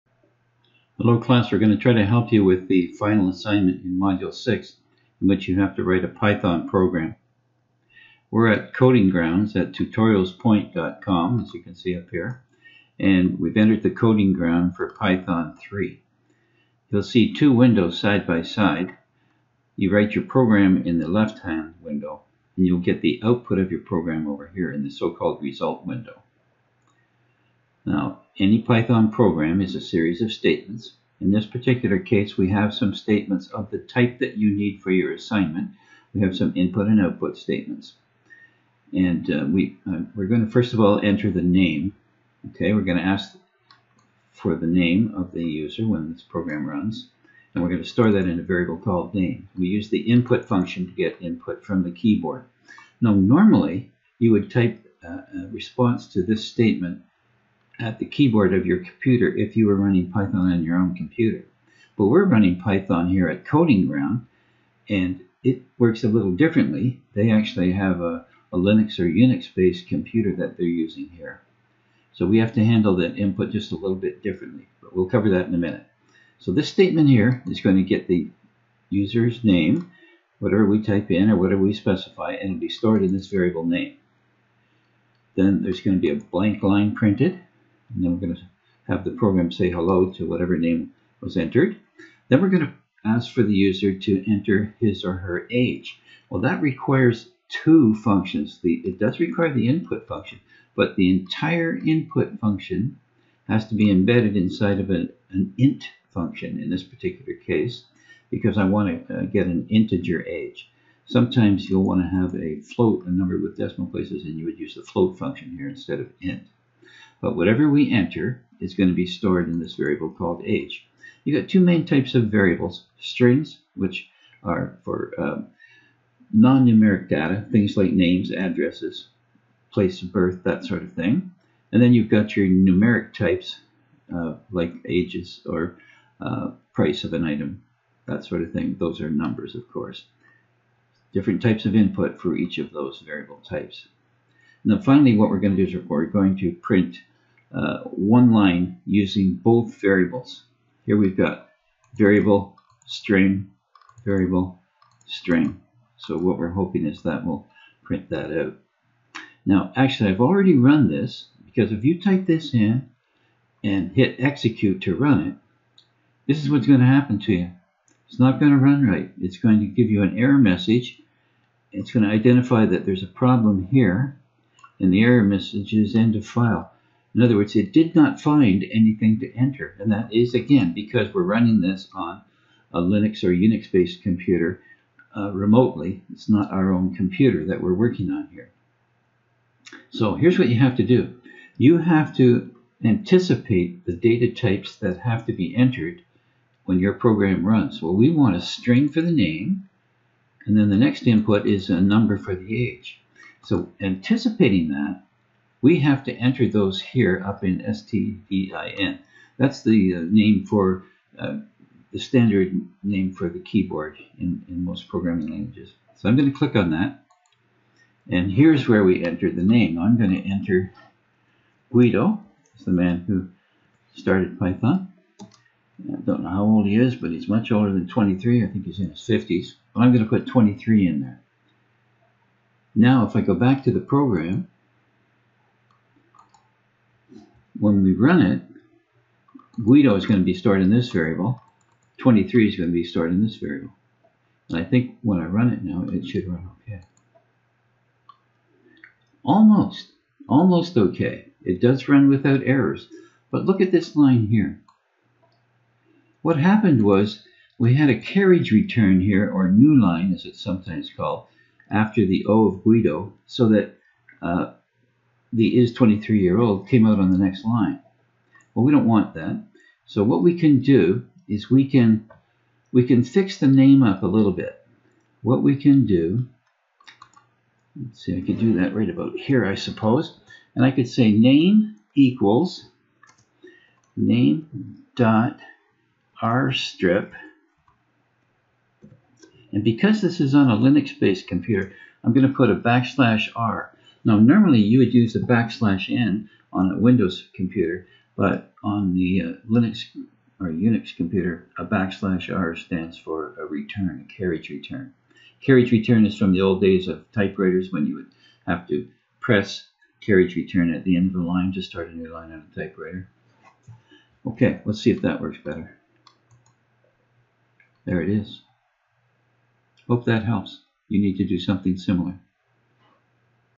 Python Online
How to use the Python Coding Ground on tutorialspoint.com